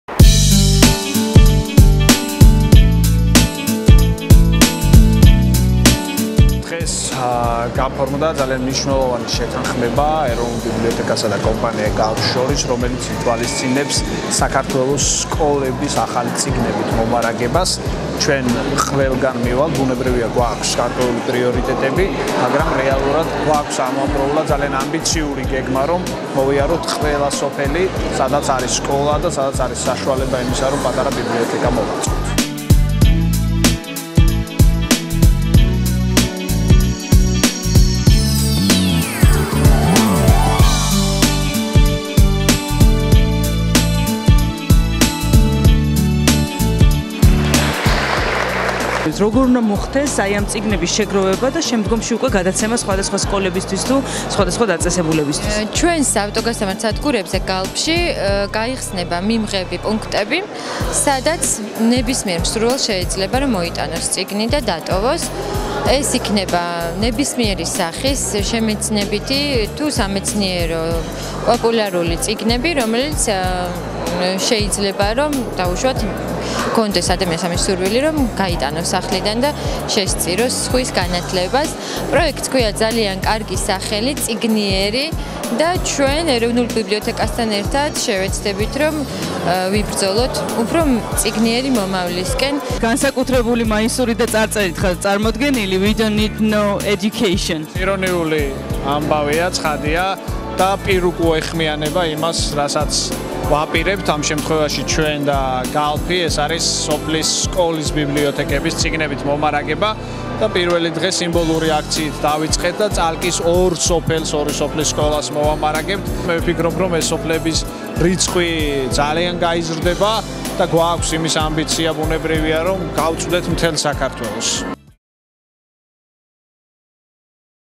წიგნიერი – ახალი წიგნები სოფლის სკოლის ბიბლიოთეკებს
გიორგიკეკელიძე : მორიგი ისტორიული ამბავი. ეროვნულ ბიბლიოთეკასა და ეკვილიბრიუმს კომპანია გალფი შემოურთდა და ერთობლივი ძალისხმევით სოფლის სკოლის ბიბლიოთეკებს განვაახლებთ. კამპანია წიგნიერი ხვალიდანვე დაიწყება და წალკის რაიონის ორ სოფელს ვესტუმრებით!